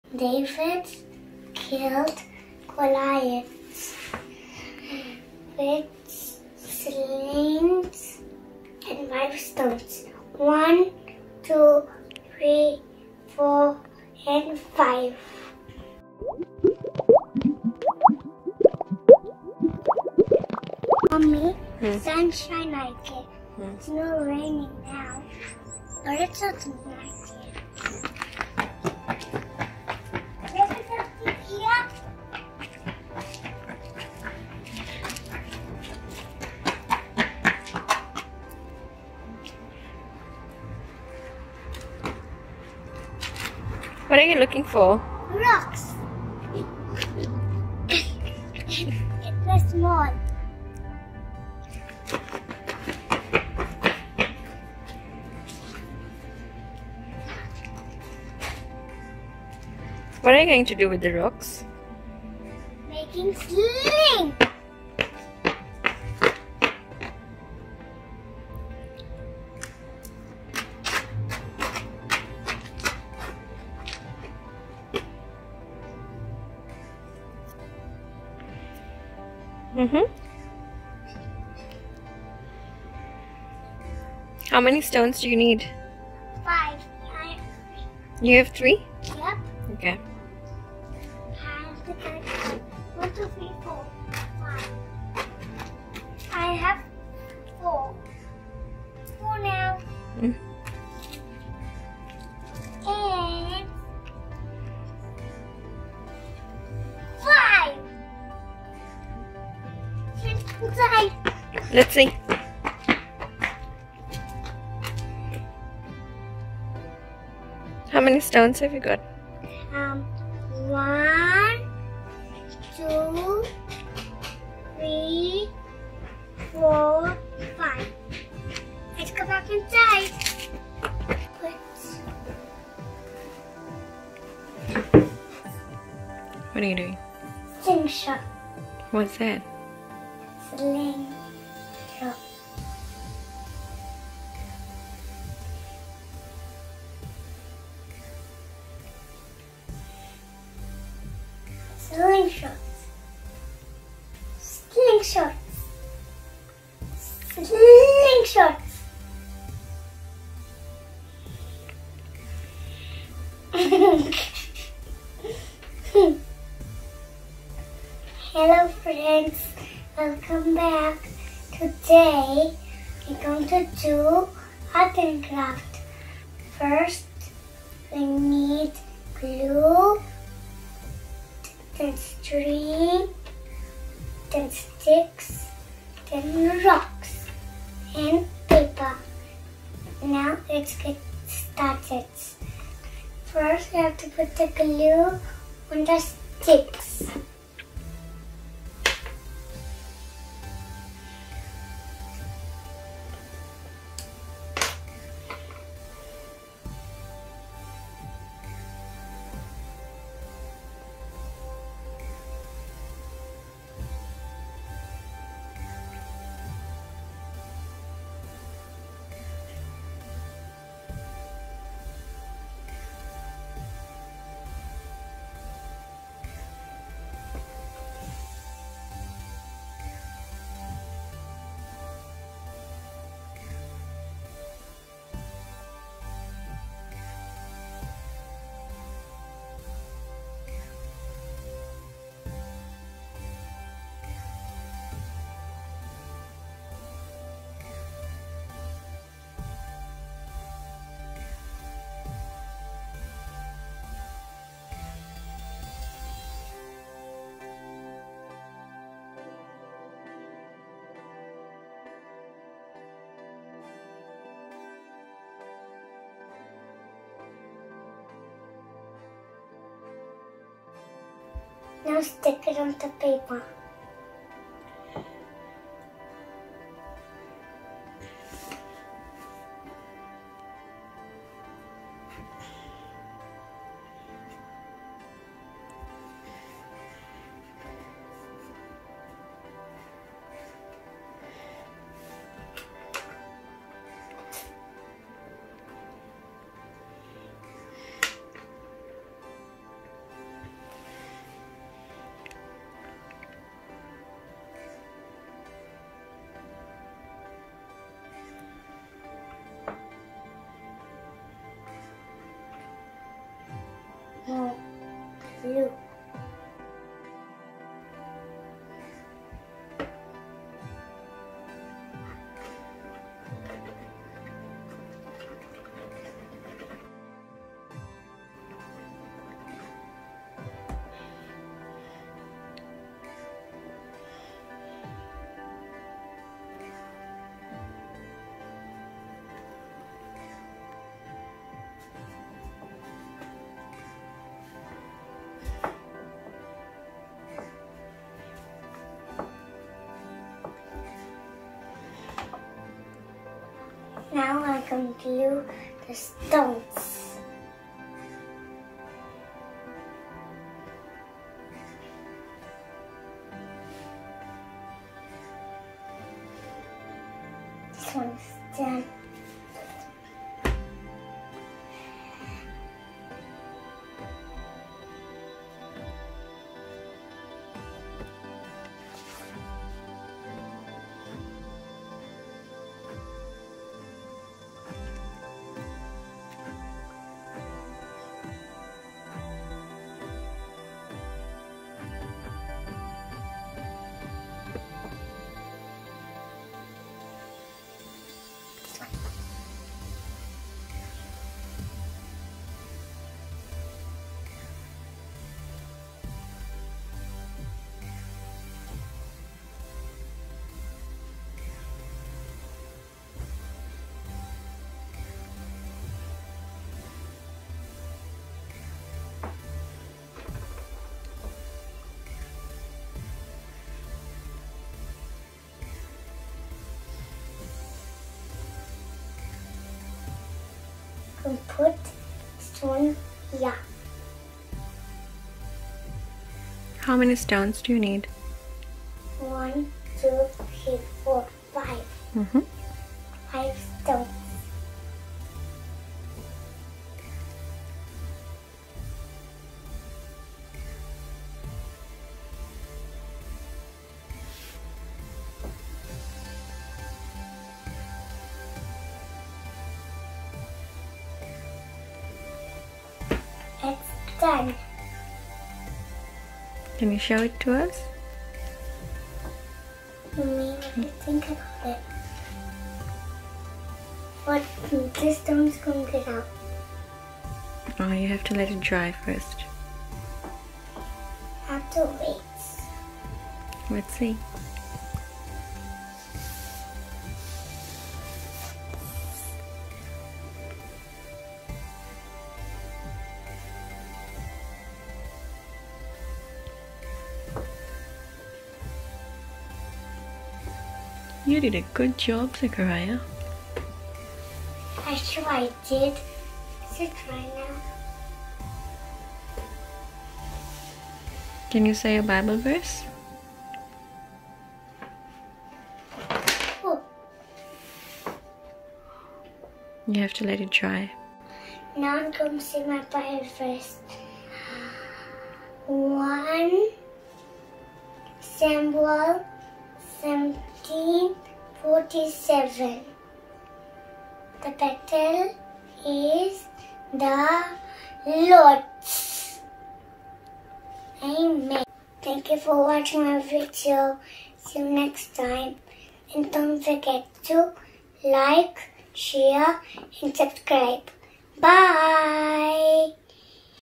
1 Samuel 17:47|Bible Verse|Zechariah Reviews|Sunday School Lesson
David accepts the challenge. Saul reluctantly agrees and offers his armor, which David declines, taking only his staff, sling and five stones from a brook. David and Goliath confront each other, Goliath with his armor and javelin, David with his staff and sling.
This is an easy Art & Craft Activity to teach the kids about the story of David and Goliath. This was a Sunday School Online lesson given by Kinoya Assembly Church which Zec attempted.